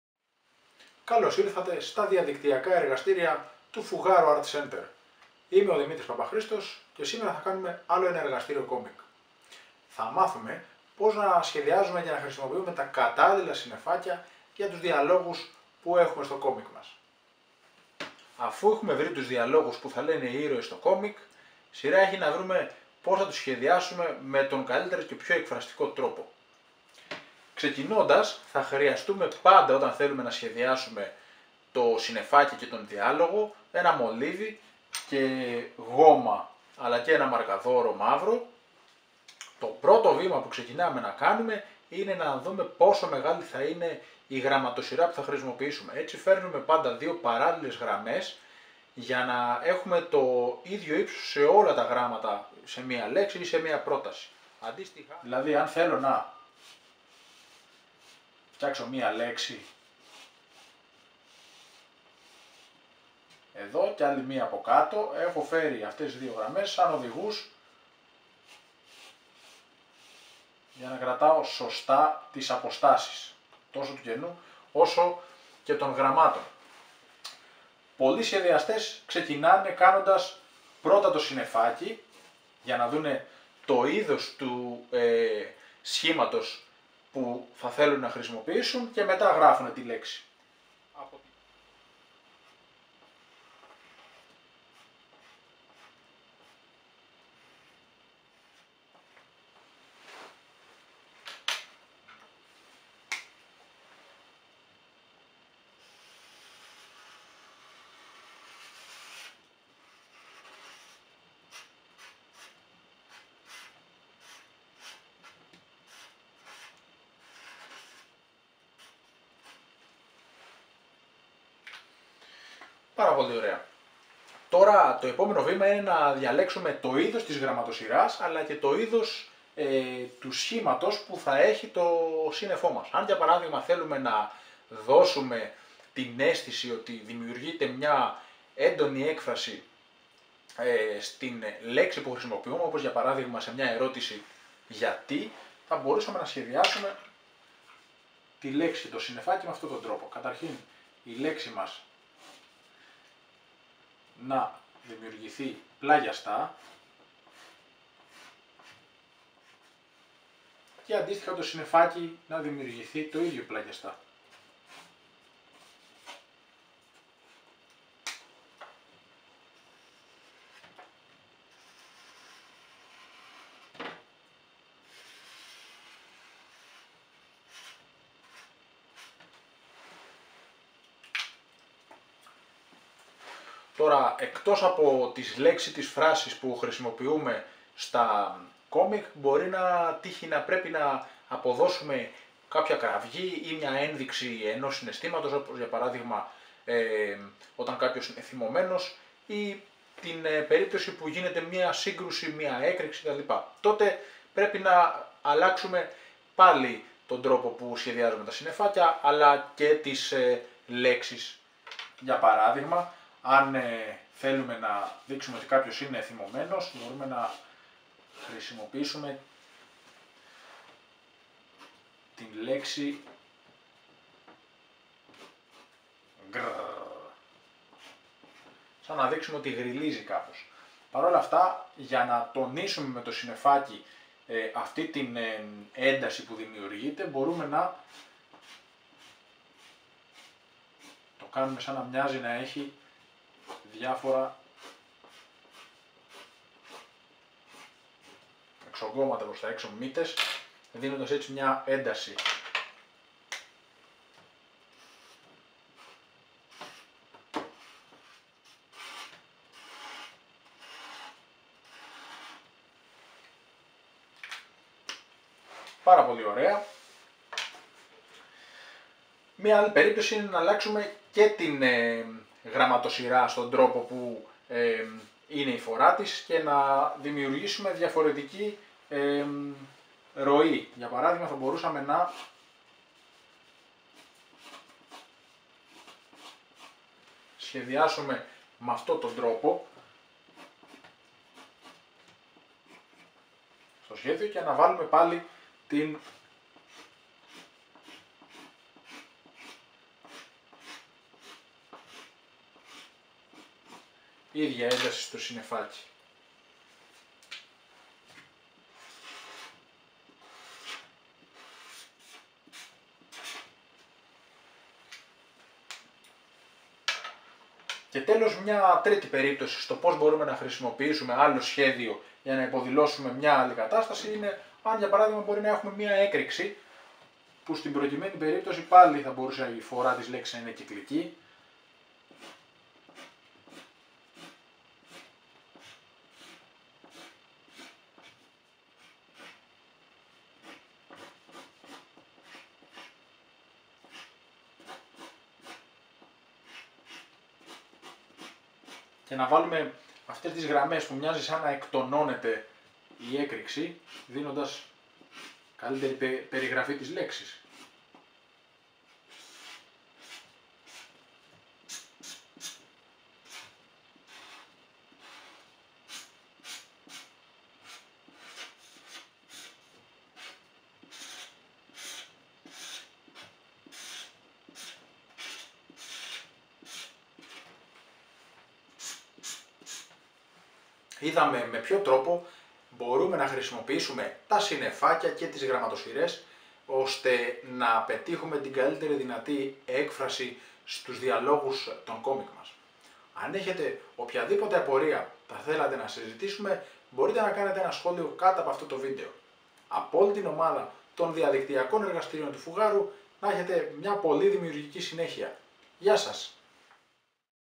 Comic | 6. Γραμματοσειρές και συννεφάκια διαλόγου
Σε αυτή την ενότητα ο Δημήτρης Παπαχρήστος μας βοηθά να ενσωματώσουμε στο σχέδιό μας συννεφάκια διαλόγου και γραμματοσειρές κειμένου.